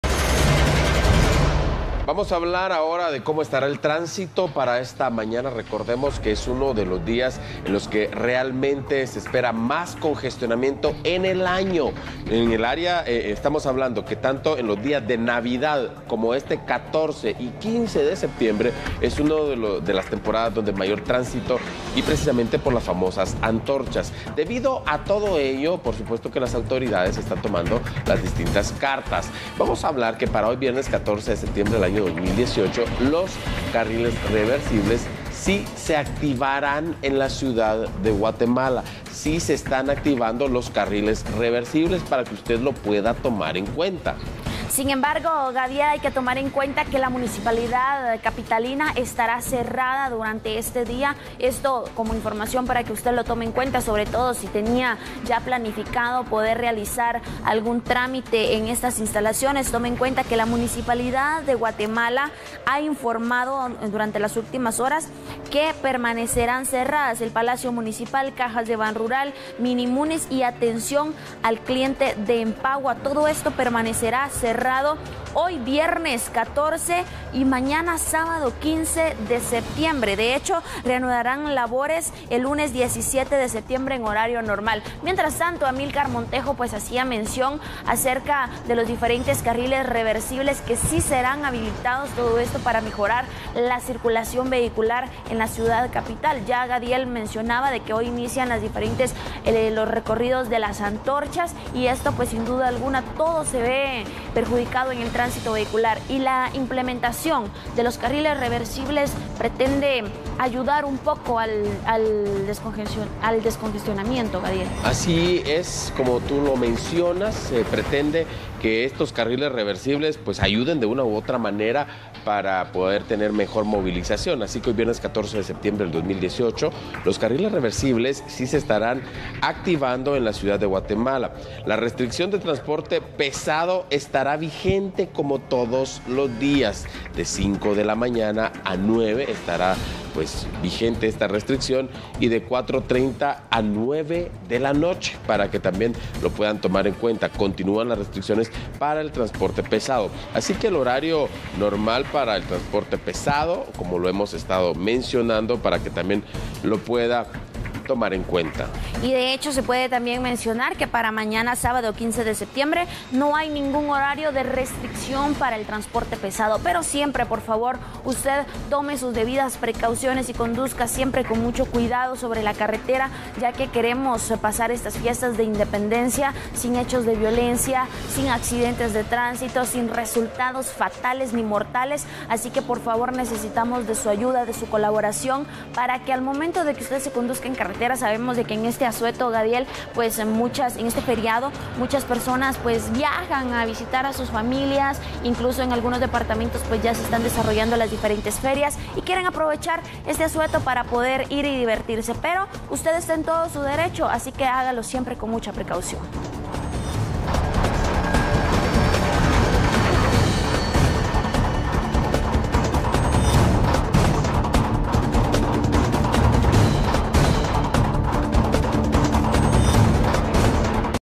Carriles reversible están con normalidad
La municipalidad capitalina estará cerrada hoy
Se estima que este viernes 14 de septiembre se uno de los días más congestionados del año por las actividades correspondientes a las fechas patrias.